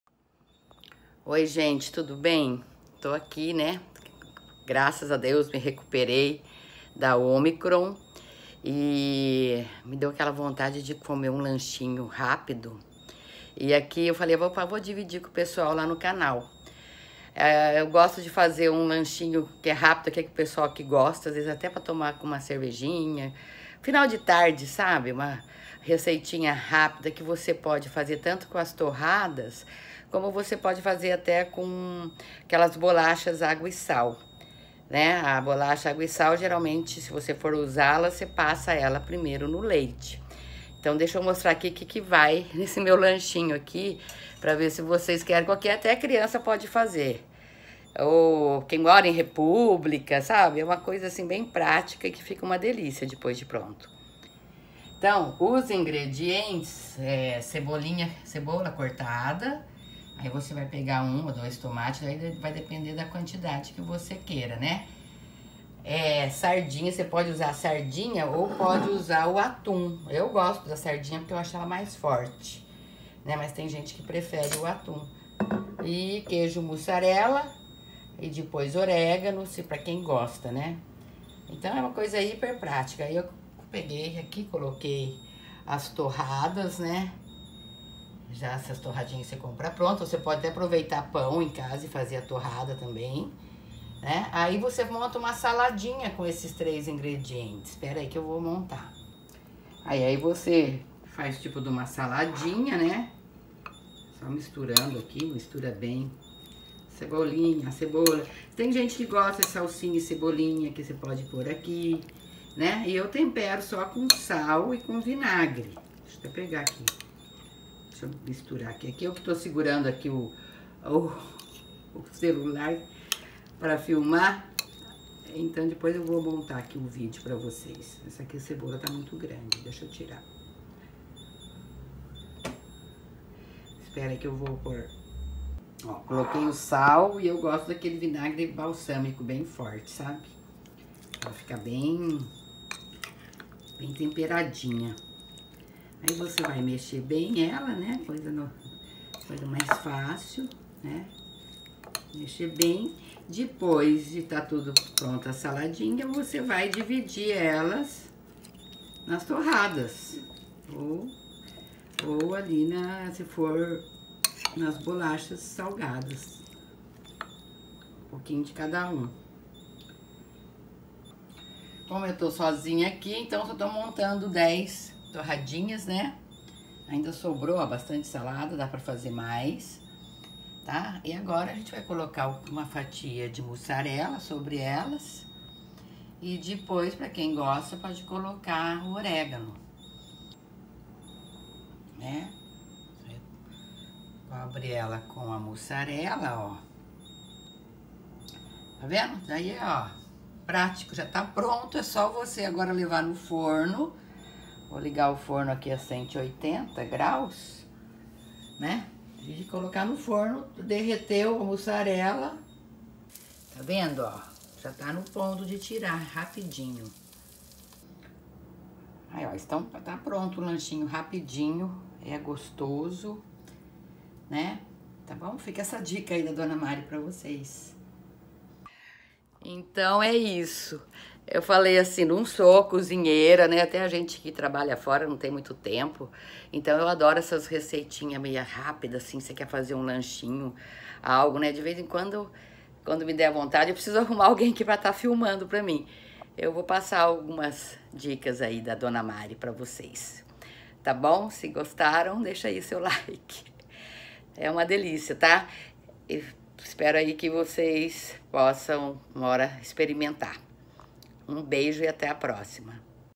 Como fazer um lanchinho rapido, gostoso e barato
Como fazer um lanchinho rápido, gostoso e barato apresenta uma receitinha basica para um lanche rapido, para você saborear a tarde.

Ingredientes:
Torradas ou bolacha agua e sal
Leite
Sardinha ou atum
Tomate
Cebola
Mussarela
Oregano

No video você verá como mentei, levei ao forno e rapidinho fica pronto.

Era isso por hoje.
Obrigada pela visita aqui no nosso canal. Você é sempre bem vindo(a) por aqui.
Um abraço